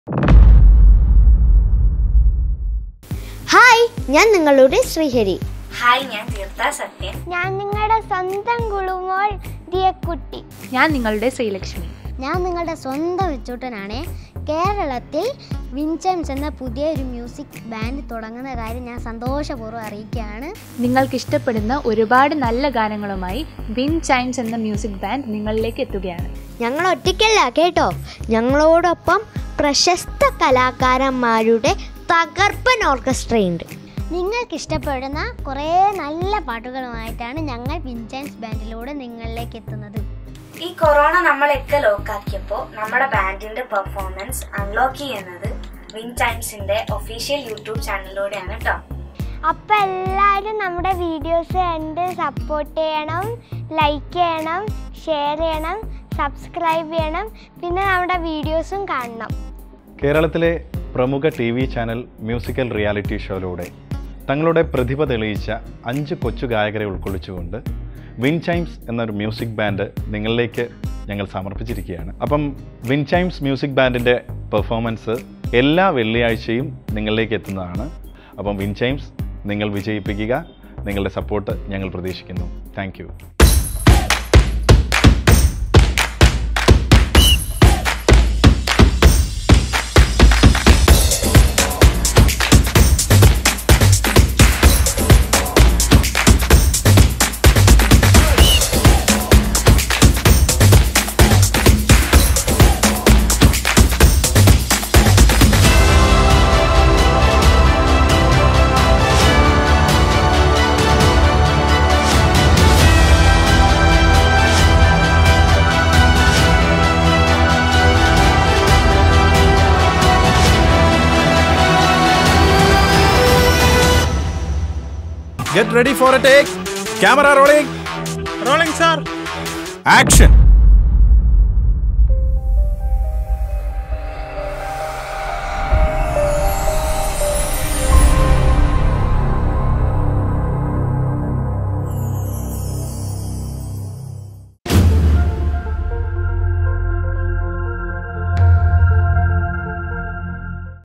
WINDCHIMES Music Band Curtain Raiser
Watch Windchimes Music Band Curtain Raiser
Singers : Sreehari | Sreelakshmi | Rithuraj | Theertha Sathyan | Krishnadiya
Show Director : Regilesh Starvoice
Camera Team :Praviraj V Nair, Manu Joseph, Gireesh Pootheri, Abin Chandran
Studio Floor : Rhythm House, Calicut
Sound Engineer : Abhai Lal
Editing : Jins Joseph
Production Controller : Roopesh Mavoor
Art : Ramesh Sivapuram
Make Up : Sony Sagar
Lights : Subahu Muthukad
Online Support :  Asneem Sait
Graphics : Nidhun Kadhak
Design : Raheem Ichoos
Cab : Renjith

Band Details-
Keyboard : Vineesh KP
Lead Guitar : Harikrishnan
Bass Guitar : Athul
Drums : Joel Jackson
Percussion : Rajeesh Kumar

DIGITAL PARTNER : MILLENNIUM AUDIOS & VIDEO VISION
|| ANTI-PIRACY WARNING ||
This content is Copyrighted to WINDCHIMES MUSIC BAND. Legal action will be taken against those who violate the copyright of the same.